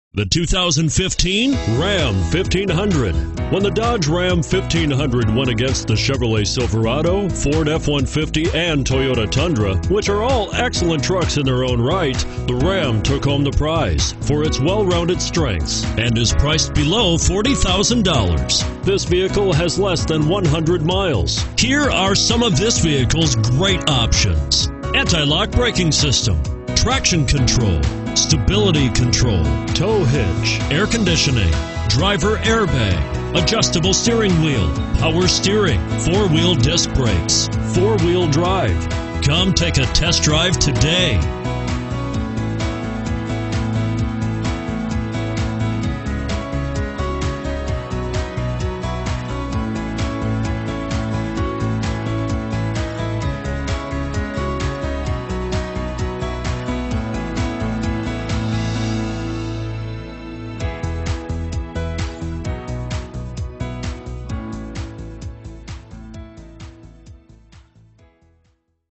2015 Ram 1500 Waterloo Iowa DT19400
2015 Ram 1500 ST - $35348

Pick up this Black Clearcoat 2015 Ram 1500, available today at Dan Deery Dodge Chrysler Jeep. Featuring Automatic transmission, it has 16 miles. This could be the one you've been searching for.